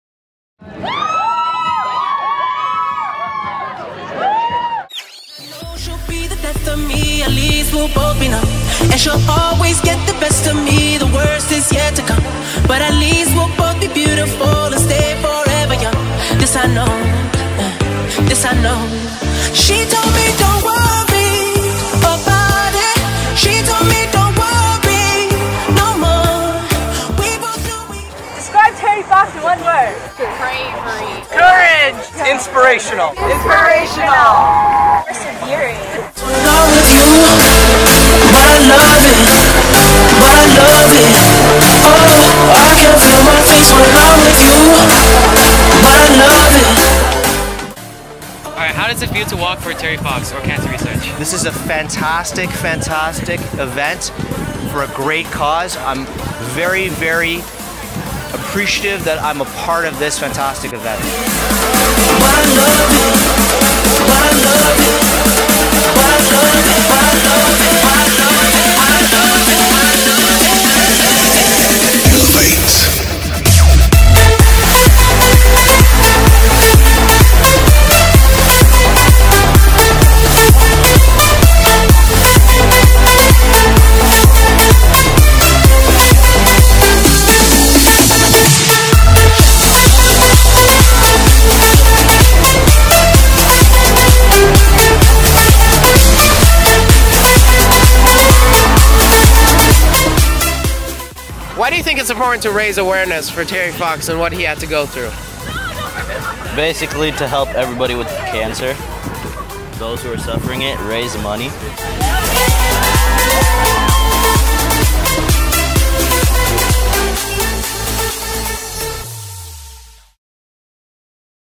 Terry Fox Run Recap 2015
St. Robert CHS's annual Terry Fox Run/Walk was, once again, an amazing event!
Thank you to everyone who participated in the run and walk, and a special thank you to SAC for organizing this successful event!
Here is a recap of the event!